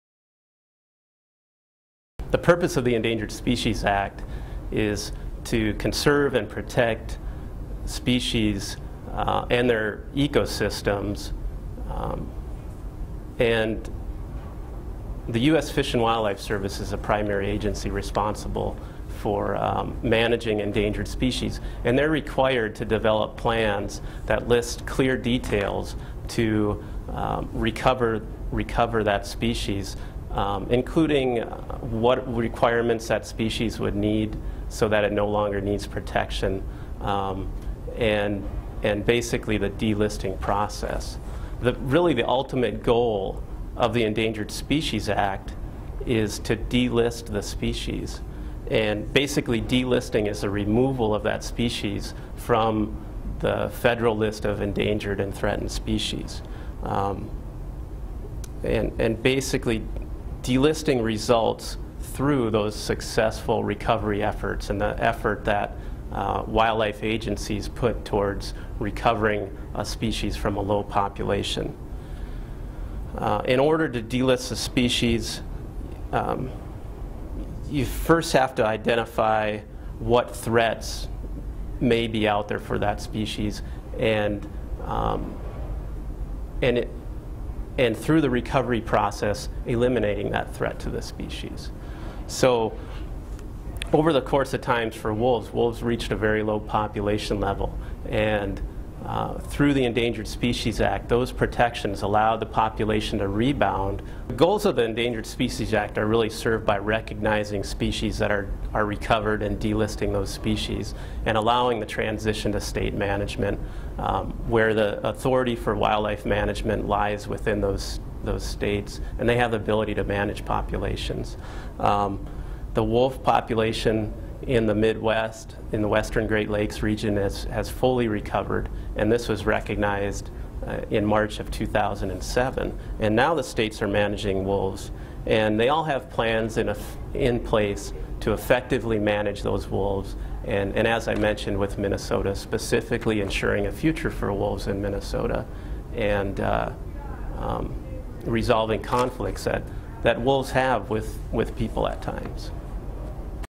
The Purpose of the Endangered Species Act
Dan Stark, wolf management specialist for the Minnesota Department of Natural Resources, talks about the purpose of the Endangered Species Act